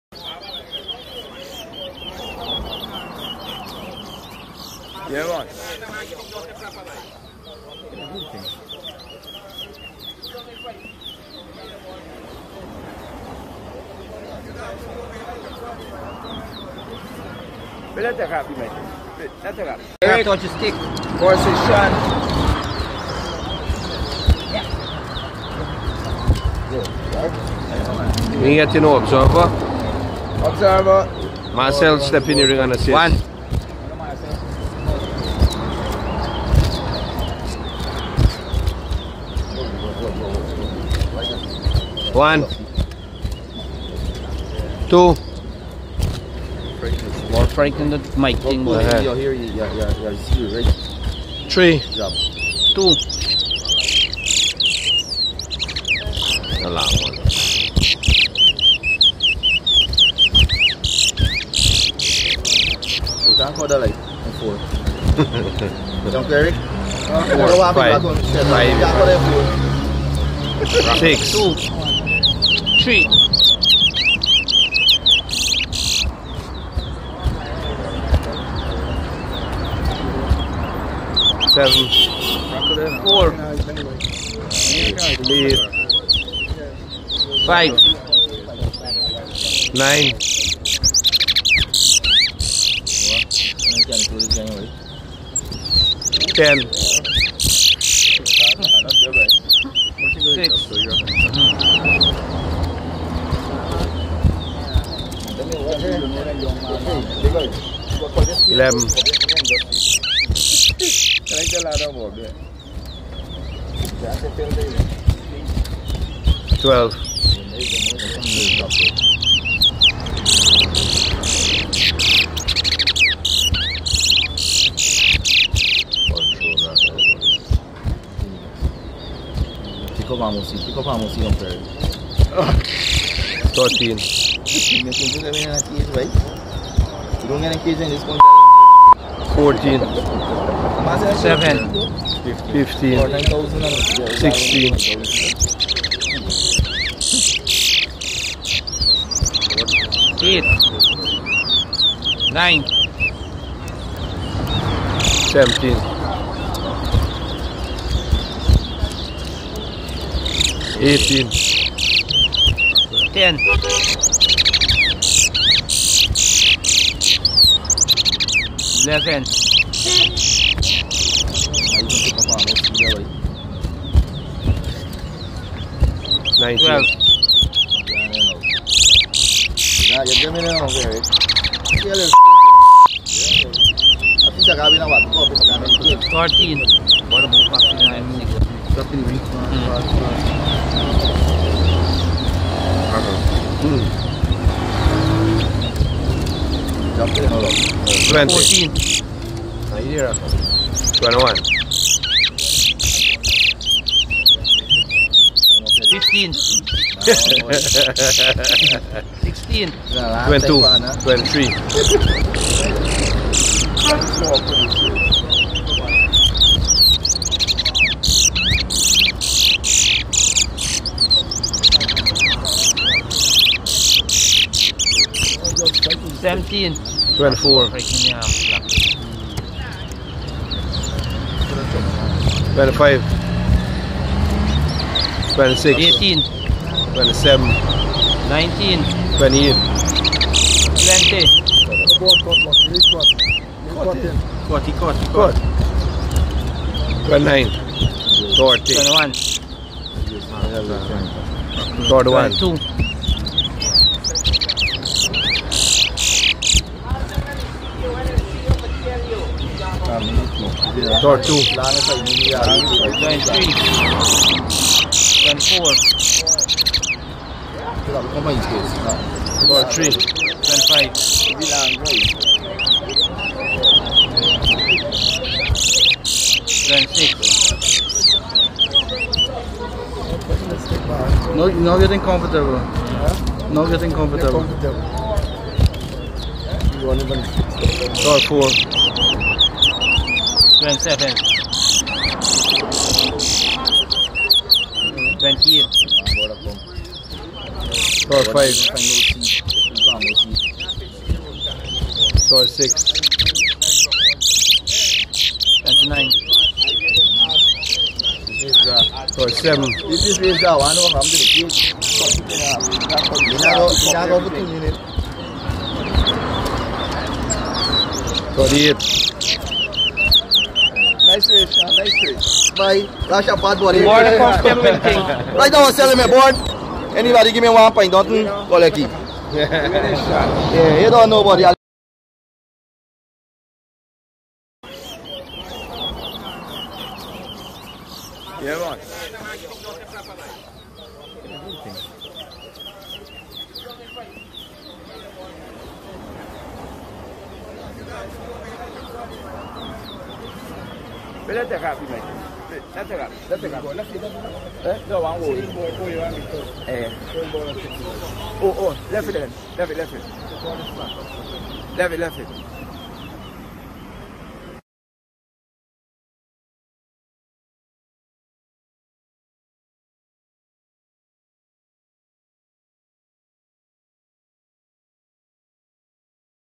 SHANIL vs ERIC🔥TODAY TOWA TOWA BIRD RACE AT ALLIGATOR POND 🇬🇾 WEST SIDE BIRD GROUND 20SHOWDOWN23 🌐
!!NO BIRDS/ANIMALS WAS HARMED IN THE MAKING OF THIS VIDEO!!

CONGRATULATIONS 🎊 SHANIL THE WINNER 🏆